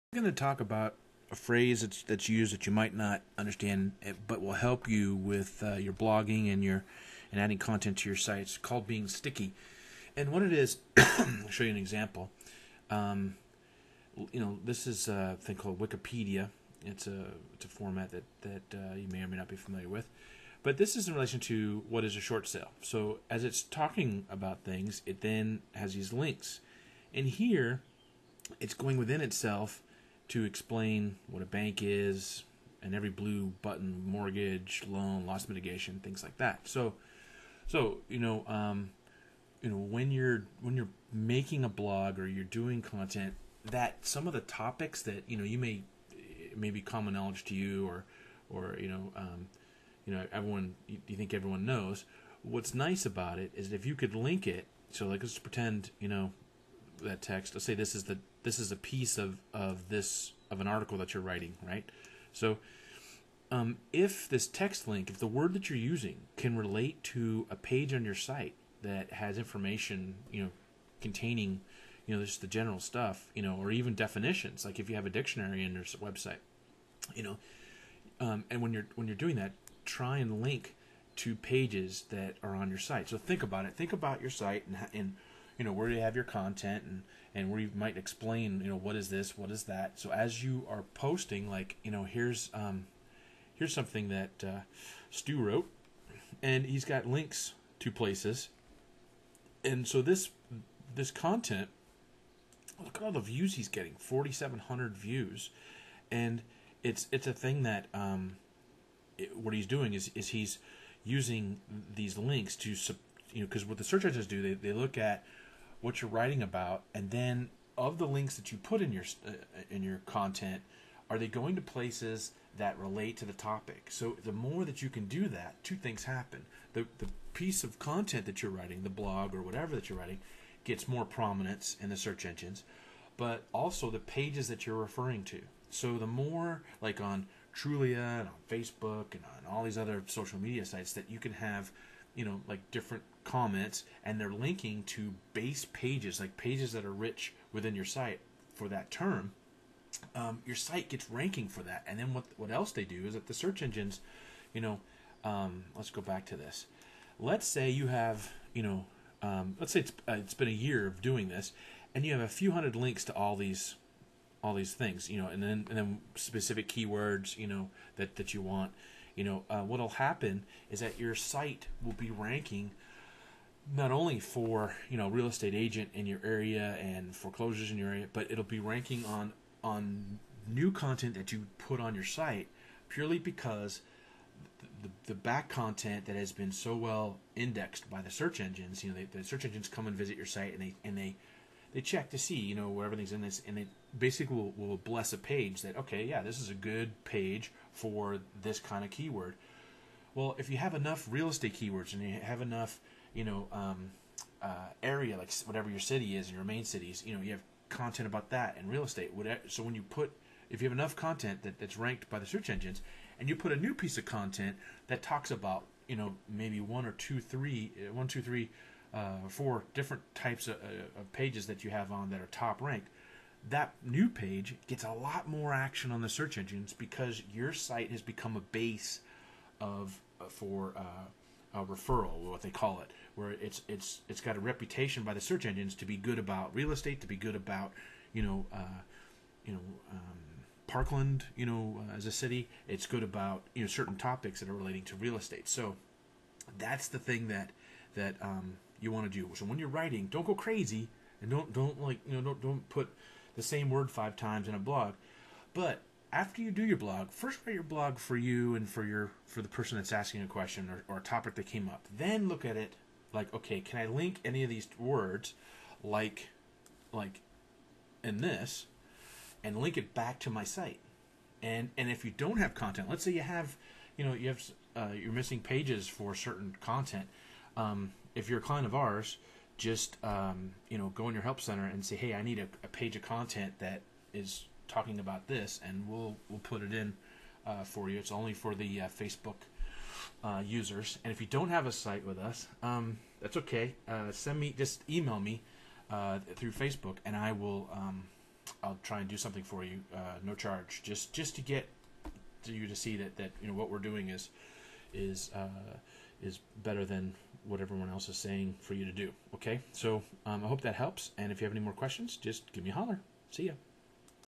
Real Estate Web Design-Making Content Sticky
Real Estate Web Design-Making Real Estate Content Sticky. Shows you how to look at your real estate site or real estate blog a new way. Once you do your writing and making it sticky a few times it will get you in a groove that will pay off for sure.